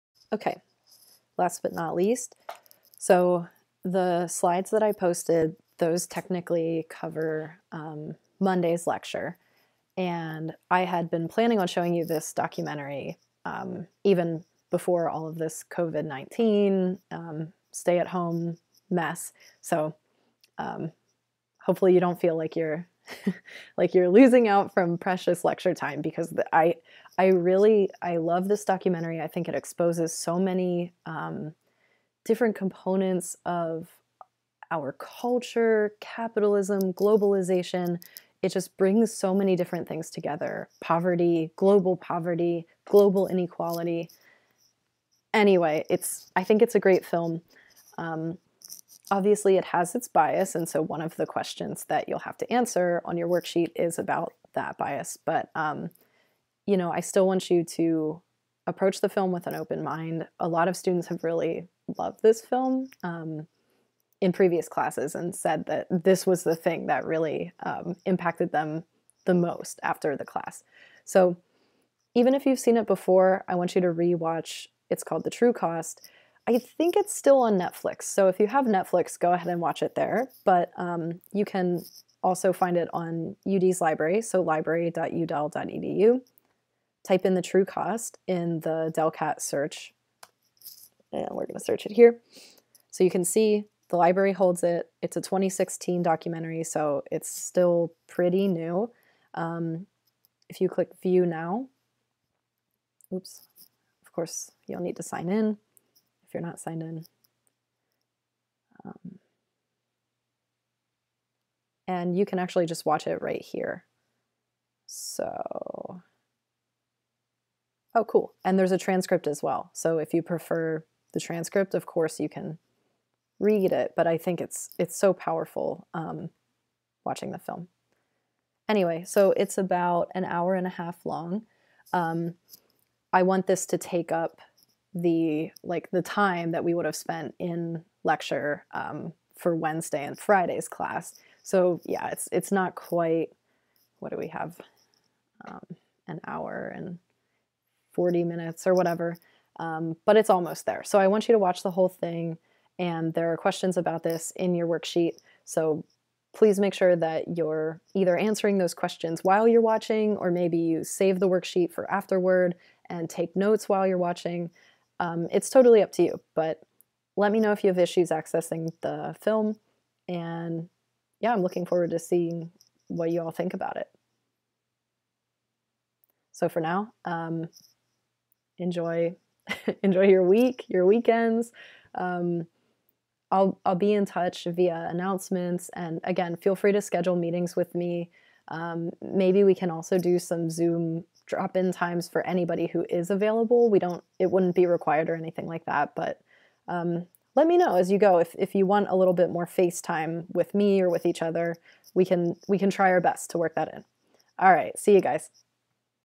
Find The True Cost doc in UD library
How to find the assigned documentary, The True Cost, in UD's library database. You can also search Netflix to see whether it's still available there.